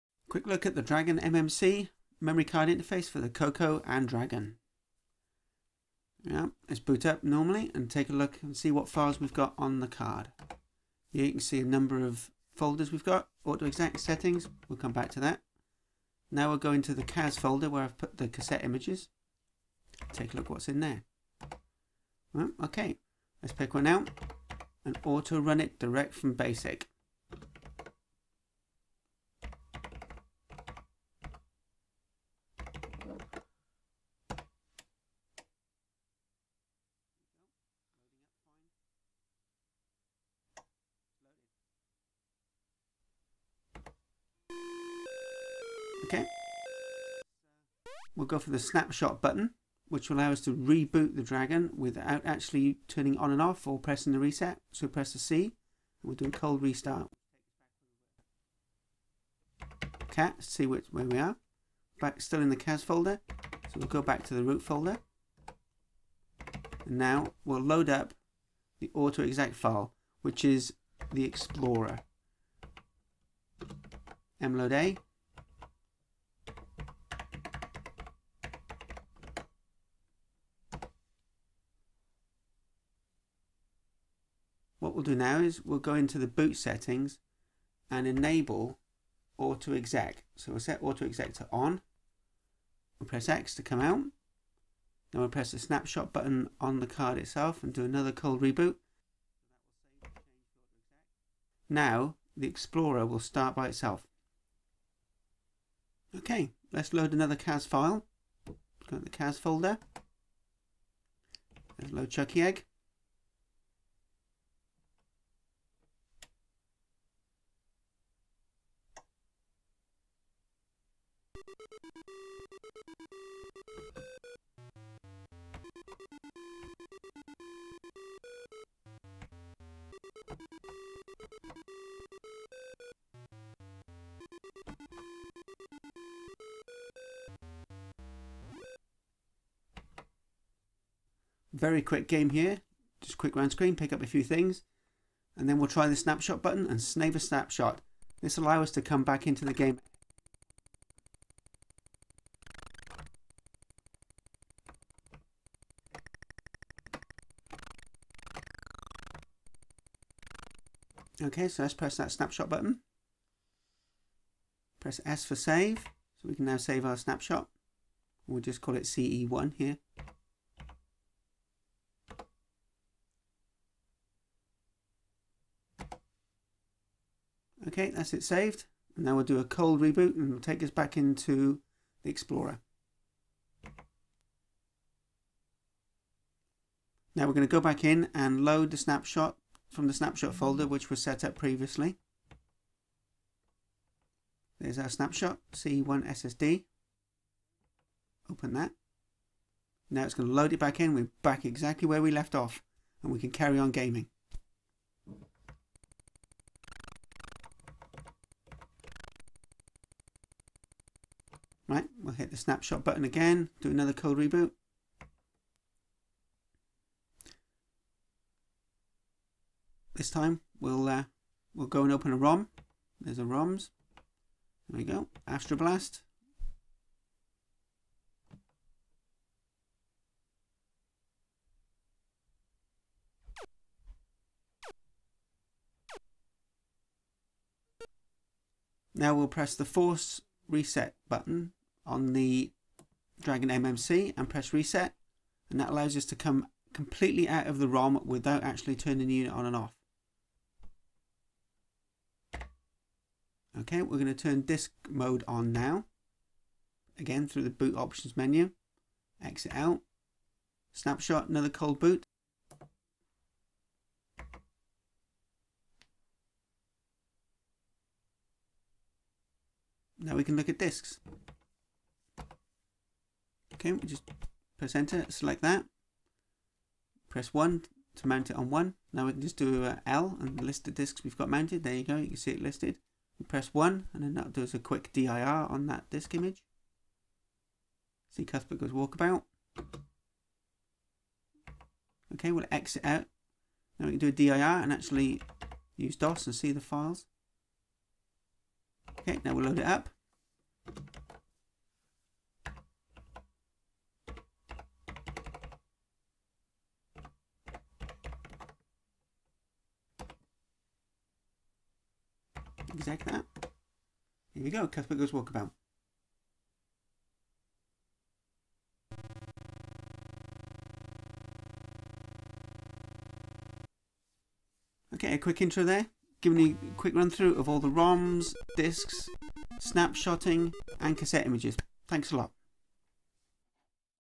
Quick Look at the Dragon DMMC and Explorer
DragonMMC is a mass storage device for the Dragon 32, Dragon 64 and Tandy CoCo 1 or 2 that provides fast access to files stored on an MMC or SD card.

This intro shows loading cassette, ROM and disks images via BASIC and Explorer.